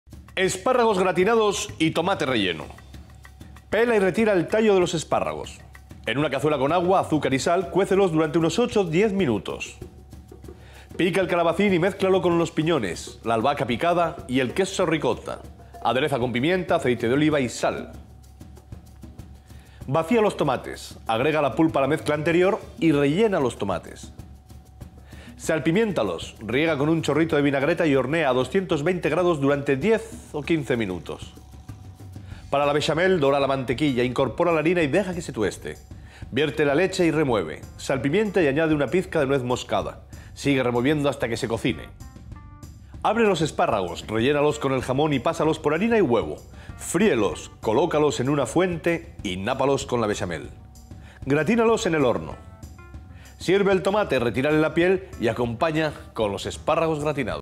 Receta de Espárragos gratinados y tomate relleno de Bruno Oteiza
Bruno Oteiza prepara una receta de espárragos rellenos de jamón con bechamel gratinada y tomate relleno de calabacín, piñones, queso ricotta y albahaca. RECETA